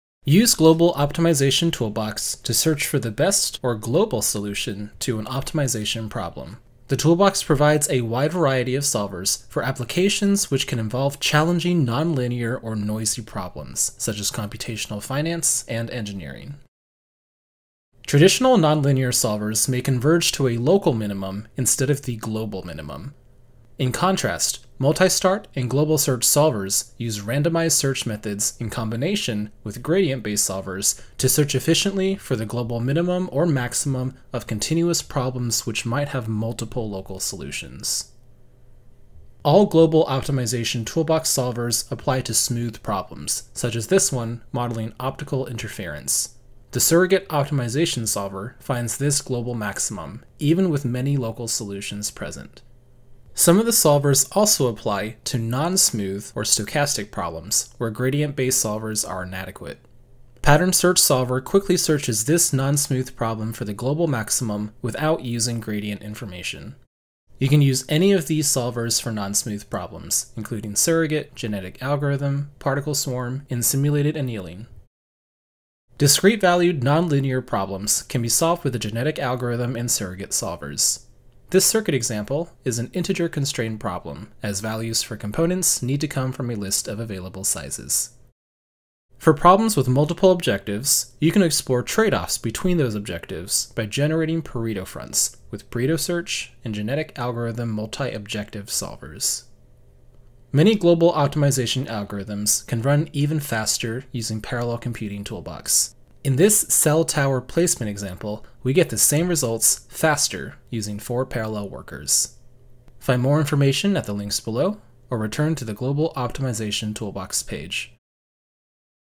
What is Global Optimization Toolbox?
Global Optimization Toolbox provides functions that search for global solutions to problems that contain multiple maxima or minima. Toolbox solvers include surrogate, pattern search, genetic algorithm, particle swarm, simulated annealing, multistart, and global search. You can use these solvers for optimization problems where the objective or constraint function is continuous, discontinuous, stochastic, does not possess derivatives, or includes simulations or black-box functions. For problems with multiple objectives, you can identify a Pareto front using genetic algorithm or pattern search solvers.